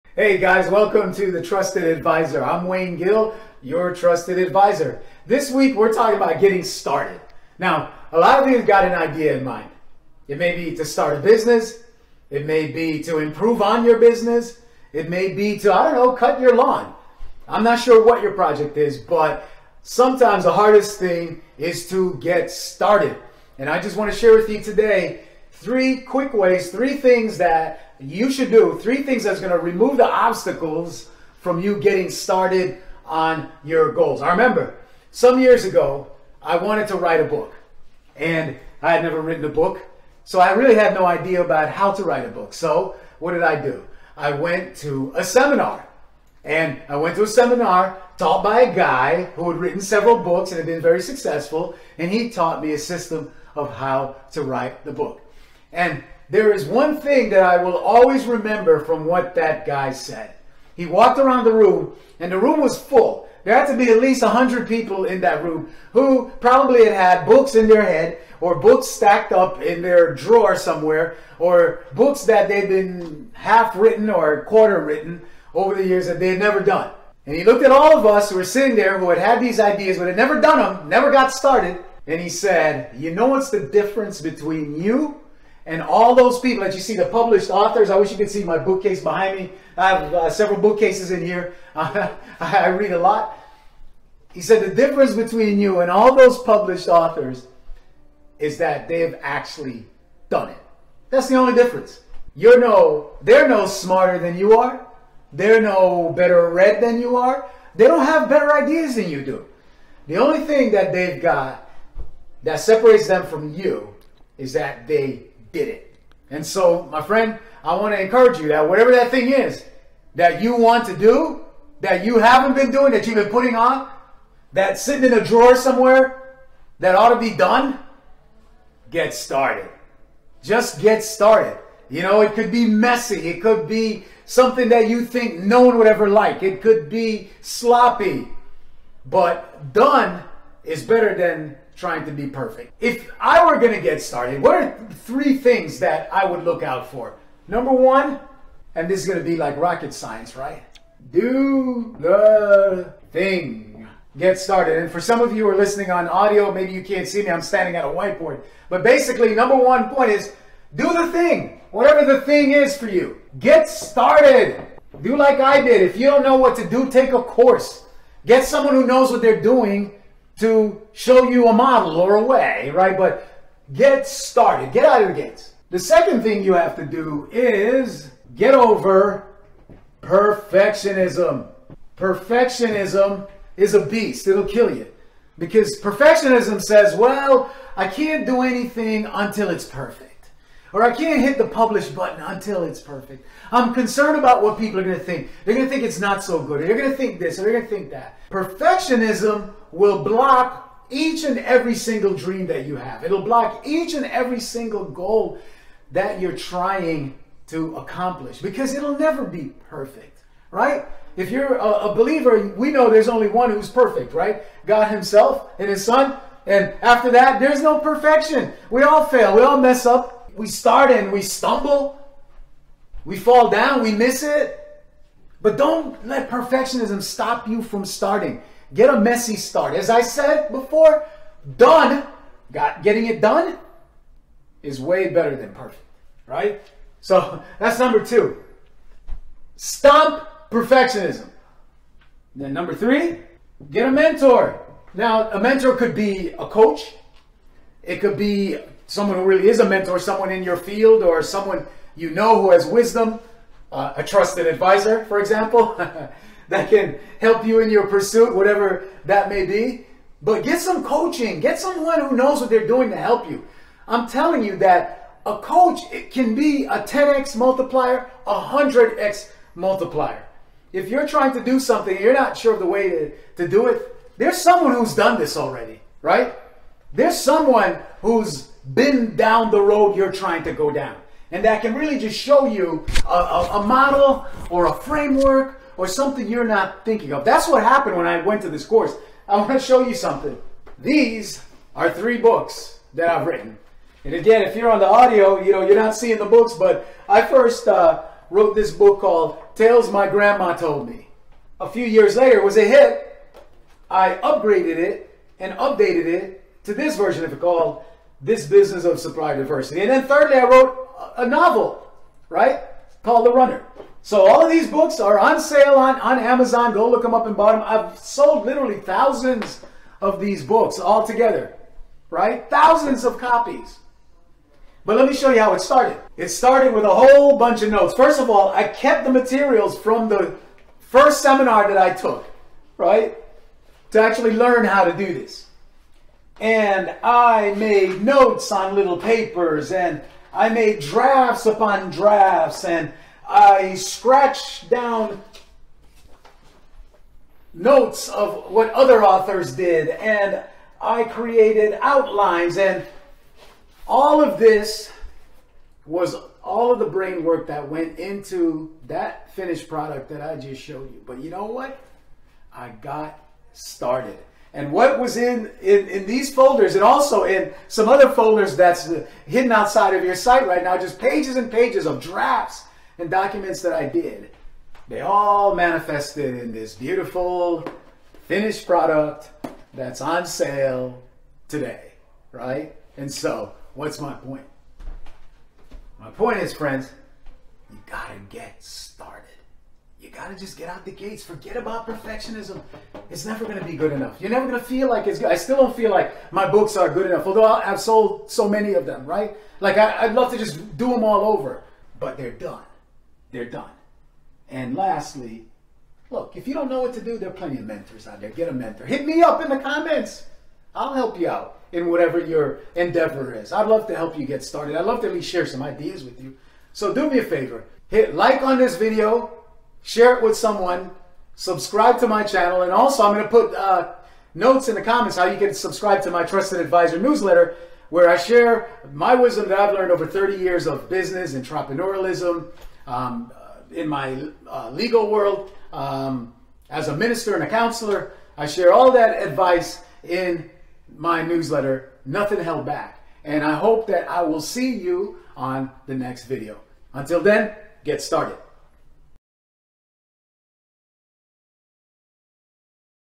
3 Steps to Get Started | Wayne Gill
Have you ever arrived to a mental roadblock when starting to pursue your passion? Here are three steps to overcome that roadblock and move on to complete your awesome project!